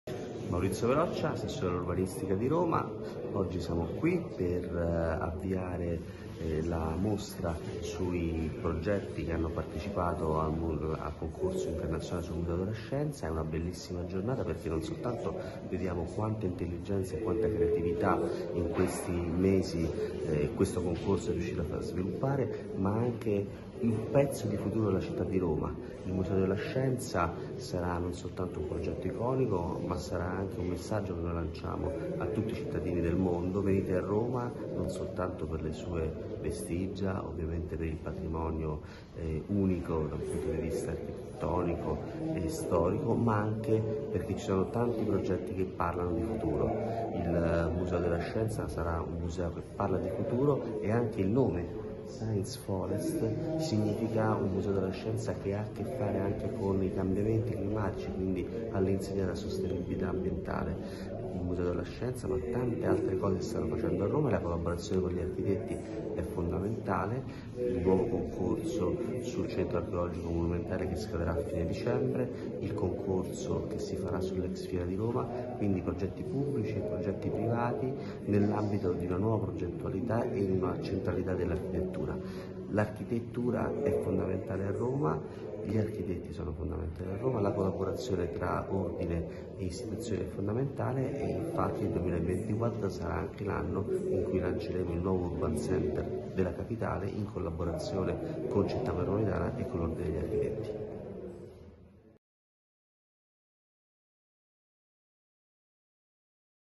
Maurizio Veloccia | Città della Scienza di Roma. Il concorso, i progetti
Maurizio Veloccia | Assessore all'Urbanistica Roma Capitale
Mostra:
📐CITTÀ DELLA SCIENZA DI ROMA
Il concorso, i progetti
🕐Dal 4 dicembre 2023 – 7 gennaio 2024
Casa dell’Architettura, Piazza Manfredo Fanti, 47 - Roma
🎯La mostra raccoglie le proposte presentate in occasione del concorso di progettazione internazionale per la realizzazione del Museo della Scienza di Roma nel contesto del processo di trasformazione urbana dell’ex Stabilimento militare materiali elettronici e di precisione di via Guido Reni quale importante nuova infrastruttura pubblica culturale dedicata alla ricerca e alla divulgazione scientifica.
Collocato tra la via Flaminia e l’ansa del fiume Tevere, un’area peculiare e in profonda trasformazione, il progetto per il nuovo Museo della Scienza di Roma è stato occasione d’indagine critica sulla valenza e il ruolo che un museo della scienza debba assumere nella città contemporanea inteso a partire dalle linee guida del programma concepito dalla Commissione tecnico scientifica presieduta dal premio Nobel prof. Giorgio Parisi.
La mostra racconta il concorso anche come occasione di sperimentazione e riflessione critica sul ruolo che il progetto contemporaneo assume in una città stratificata e complessa come Roma, ristabilendo la centralità dell’ideazione progettuale come fattore fondamentale nella visione strategica della Città.
I 70 progetti segnano il passo di un attraversamento tra molteplici prefigurazioni, conducendo il visitatore in un viaggio meraviglioso attraverso le diverse proposte e visioni.
📣ingresso libero
dal lunedì al sabato ore 10-19